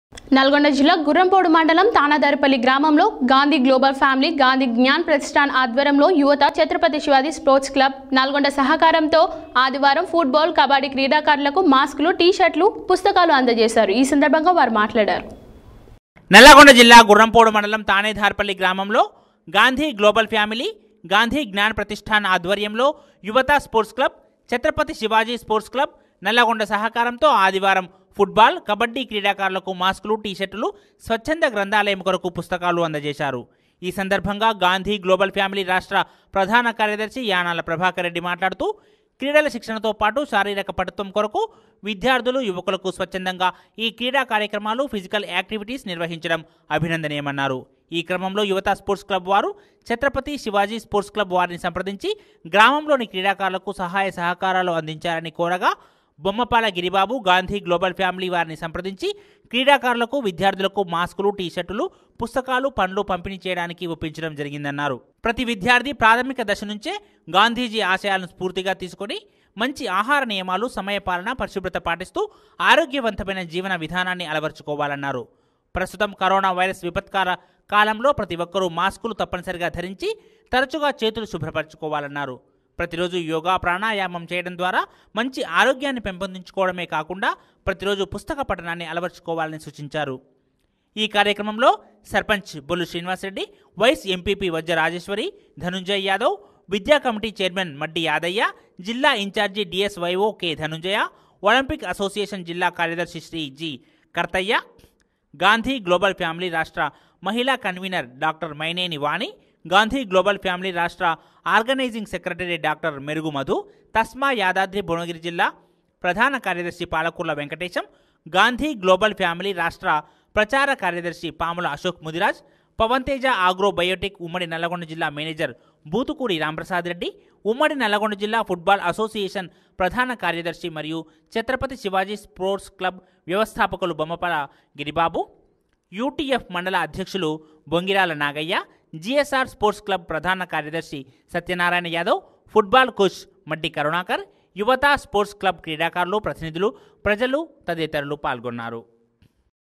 SATYA NEWS TELANGANA / 12 JULY 2020
గాంధీ గ్లోబల్ ఫ్యామిలీ గాంధీ జ్ఞాన్ ప్రతిష్ఠాన్ ఆధ్వర్యంలో .ఫుట్బాల్ కబడ్డీ క్రీడాకారులకు మాస్కులు టీ షర్ట్లు పంపిణీ